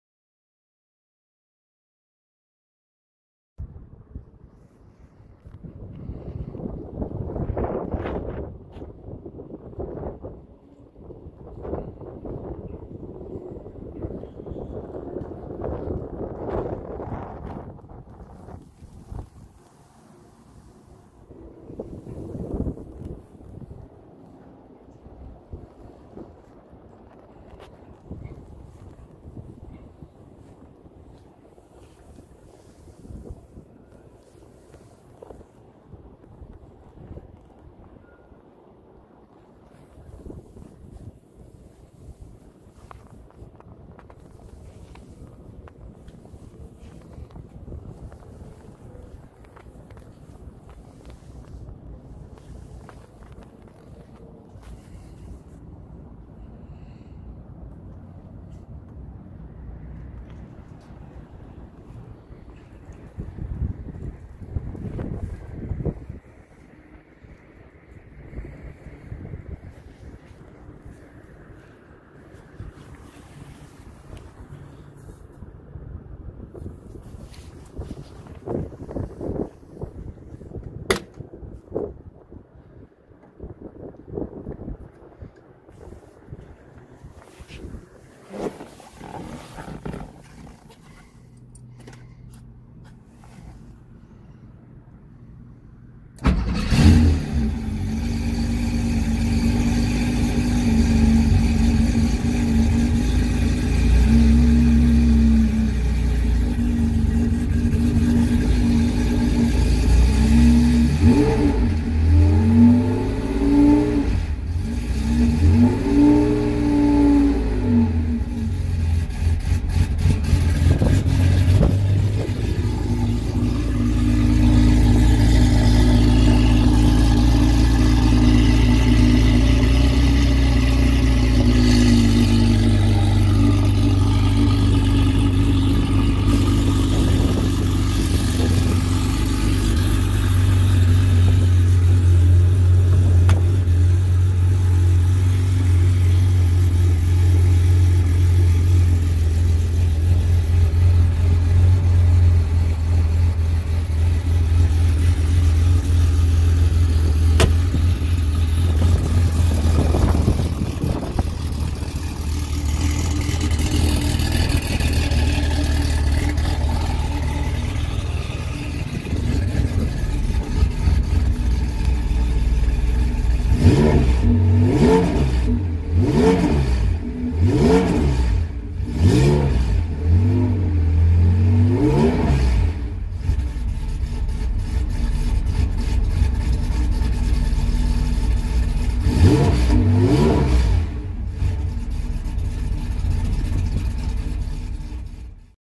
Backdated 911 Cold Start and Walk Around!
Now live on Bring a Trailer